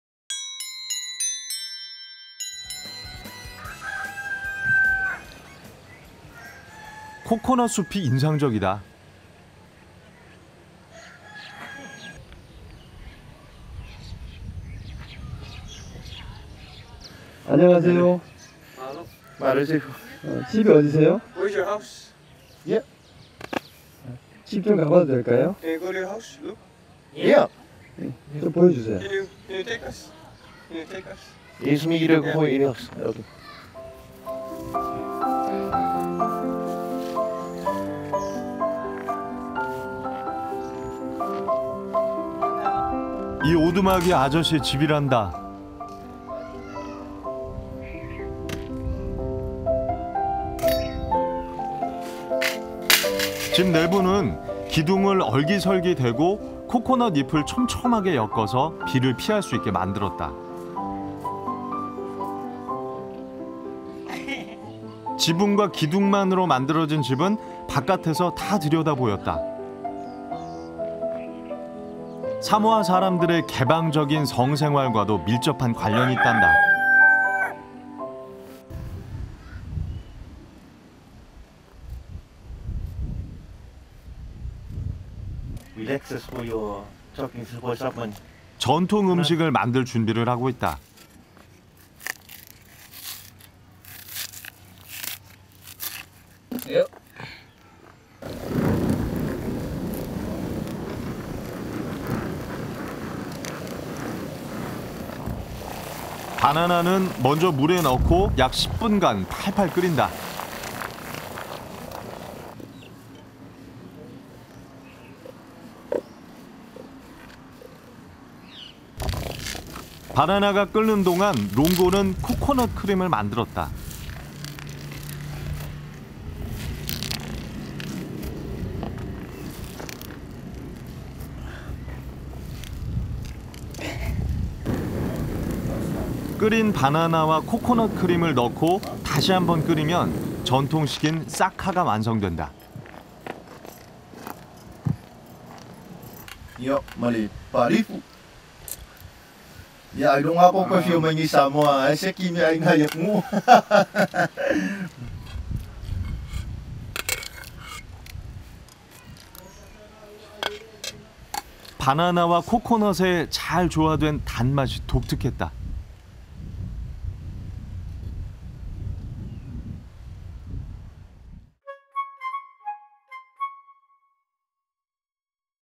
【K】Samoa Travel-Apia[사모아 여행-아피아]코코넛 집 팔레와 바나나 요리 싸카/Fale House/Coconut Forest/Food/Banana/Saka
[한국어 정보]
코코넛 숲이 인상적이다. “안녕하세요?” “네, 안녕하세요.” “집이 어디세요?” “네, 저 집입니다.” “집 구경 좀 시켜주세요.” “네, 물론이죠. 따라오세요.” 이 오두막이 아저씨의 집이란다. 집 내부는 기둥을 얼기설기 대고 코코넛 잎을 촘촘하게 엮어서 비를 피할 수 있게 만들었다. 지붕과 기둥만으로 만들어진 집은 바깥에서 다 들여다보였다. 사모아 사람들의 개방적인 성생활과도 밀접한 관련이 있단다. 전통음식을 만들 준비를 하고 있다. 바나나는 먼저 물에 넣고 약 10분간 팔팔 끓인다. 바나나가 끓는 동안 롱고는 코코넛 크림을 만들었다. 끓인 바나나와 코코넛 크림을 넣고 다시 한 번 끓이면 전통식인 싸카가 완성된다. “사모아 사람들이 자주 먹는 음식입니다.” 바나나와 코코넛의 잘 조화된 단맛이 독특했다.

Coconut forest is impressive. "Hello?" "Yes, hello." "Where is your home?" "Yes, that house." "Let me get home visit." "Yes, of course. "The cabin yiranda home of the uncle. House interior is made so tight against the pillar eolgiseolgi coconut leaf weaving to avoid the rain. The house is made with only the roof and pillars looked into from the outside. Itdanda closely related to the opening of sexuality and people of Samoa. And it is ready to make a traditional food. Bananas are first put in the water and boil for about 10 minutes VITAL. Long logos made for the bananas boiling coconut cream. Put the boiled banana and coconut cream are completed once again boil the traditional fight car. "Samoa People often eat food." This well-balanced sweetness of the banana and coconut was unique.

[Information]
■클립명 : 오세아니아068-사모아01-06
■여행, 촬영, 편집, 원고 :  이상익PD (travel, filming, editing, writing : KBS TV Producer )